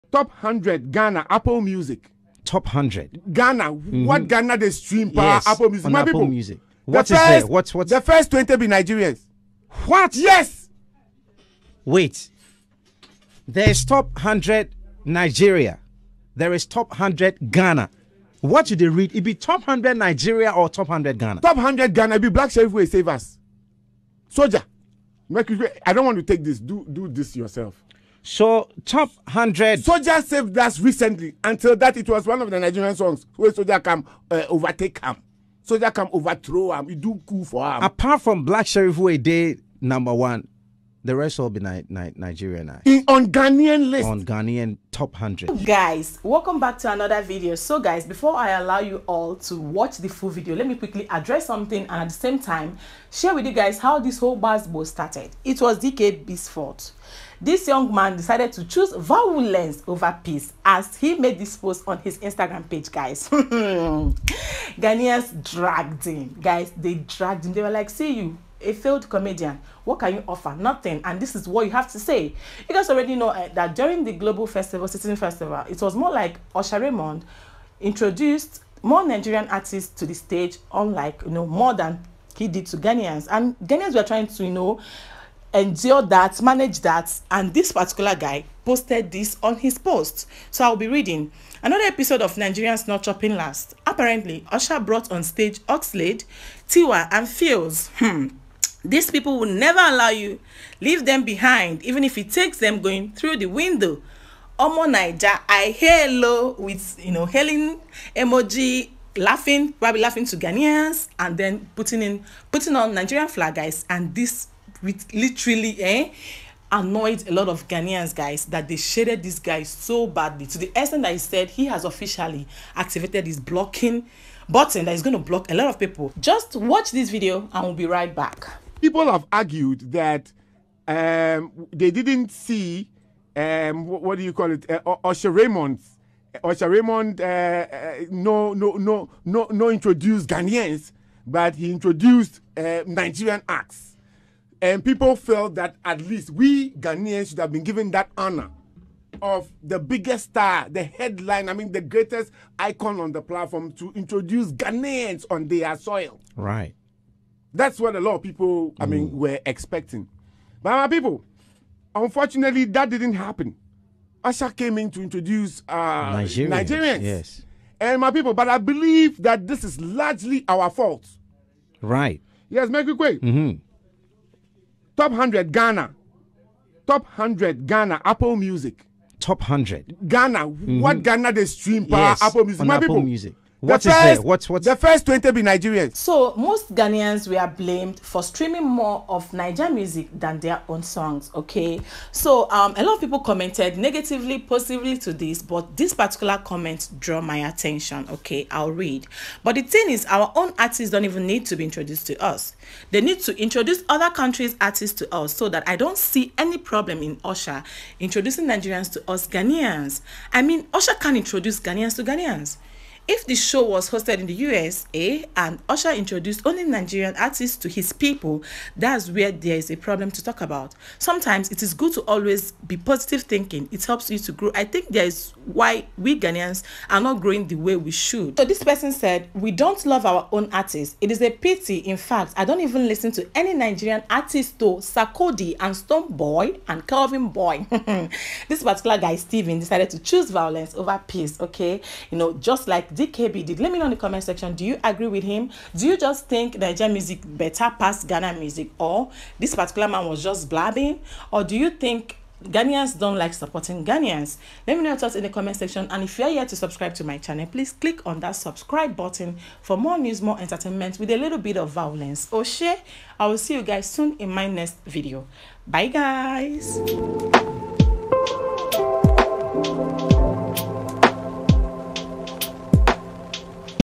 A Ghanaian Man Drags DKB & Ghanaians for Promoting Nigerian Music & Artistes More! Shame🙆🏻‍♀️🇬🇭
Watch and let me know what you think!
A lot of disagreement on the social media shortly after global citizens festival